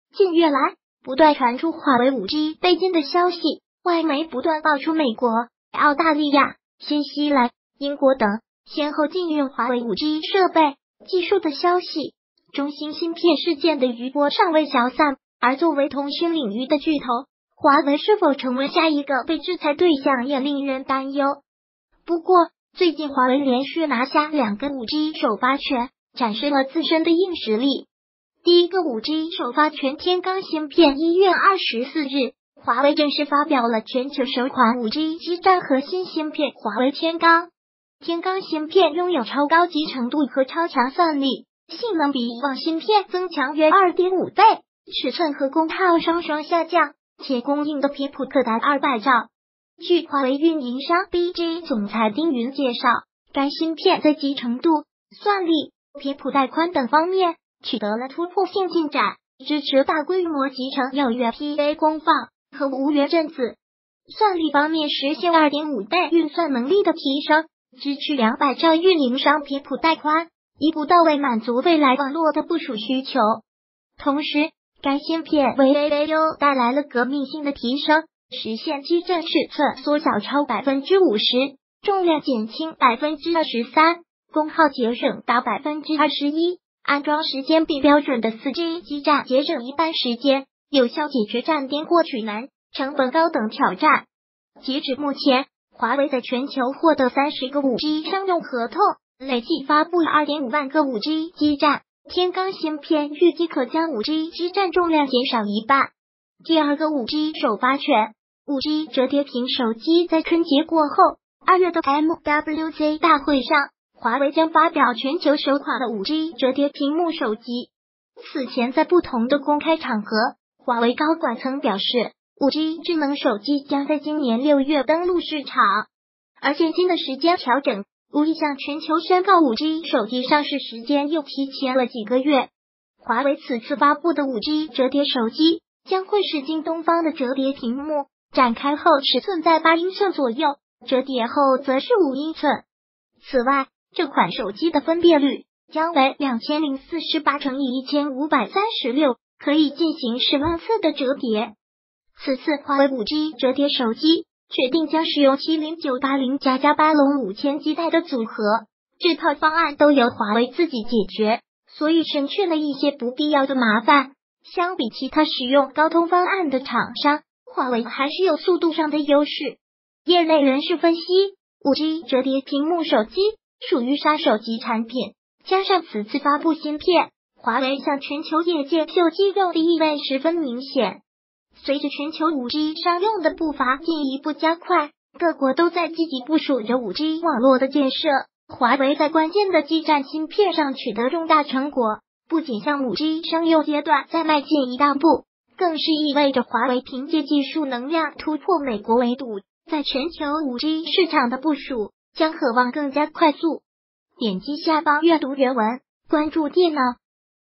华为拿下两个5G“首发权”，5G折叠屏手机真的来了
5G被禁的消息，外媒不断曝出美国、澳大利亚、新西兰、英国等先后禁用华为5G设备、技术的消息，中兴芯片事件的余波尚未消散，而作为通讯领域的巨头，华为是否成为下一个被制裁对象也令人担忧。不过，最近华为连续拿下两个5G“首发权”，展示了自身的硬实力。第一个5G“首发权”——天罡芯片 1月24日，华为正式发表了全球首款5G基站核心芯片──华为天罡。天罡芯片拥有超高集成度和超强算力，性能比以往芯片增强约2.